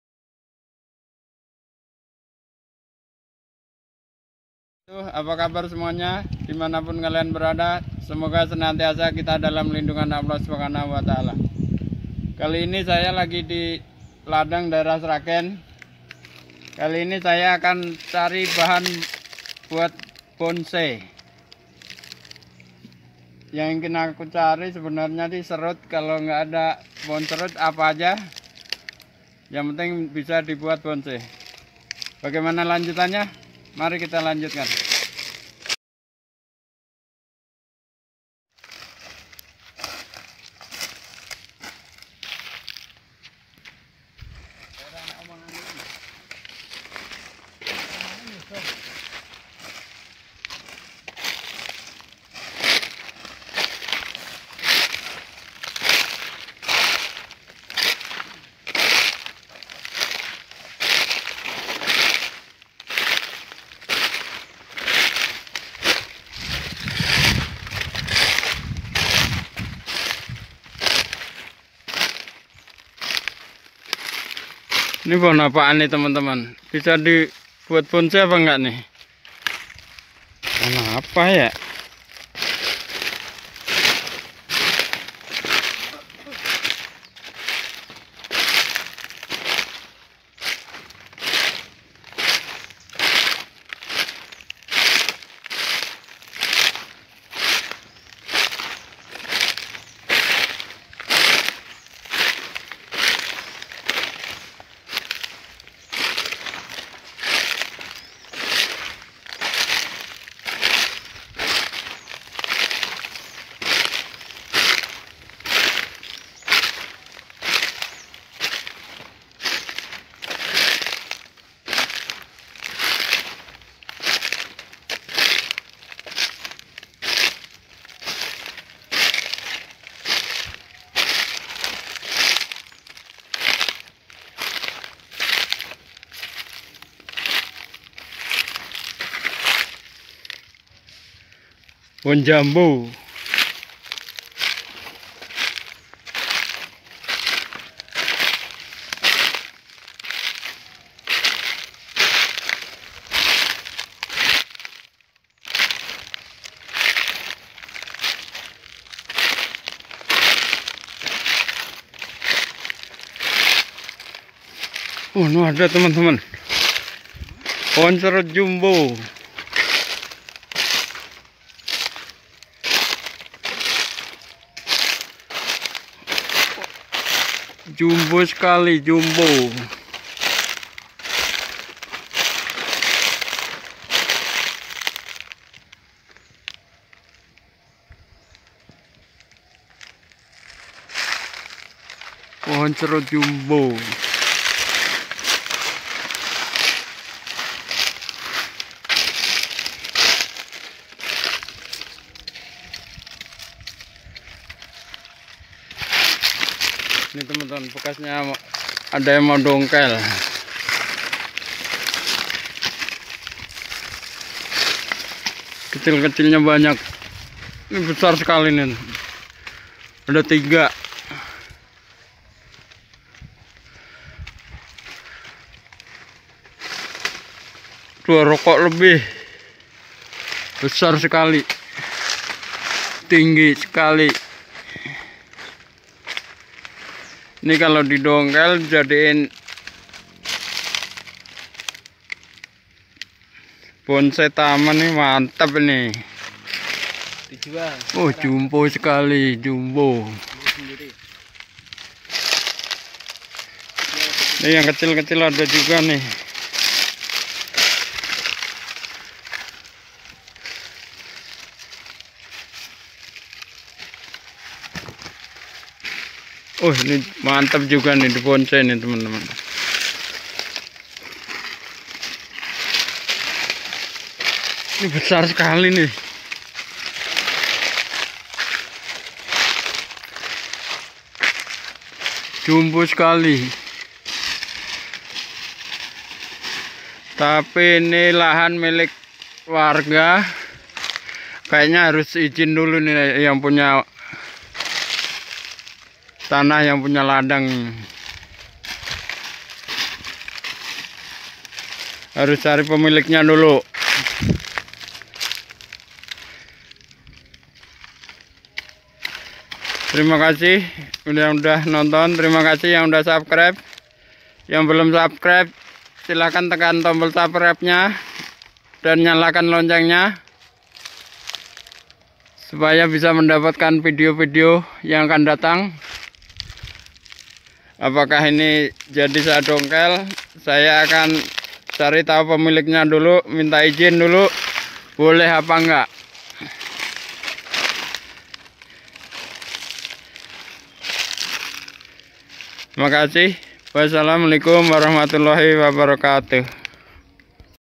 BERBURU BAHAN BONSAI SERUT
Ini pohon serut yg ukurannya sangat besar sekali, tapi saya tidak berani dongkel, karena ini ladang orang, sementara saya mau cari dulu siapa pemiliknya.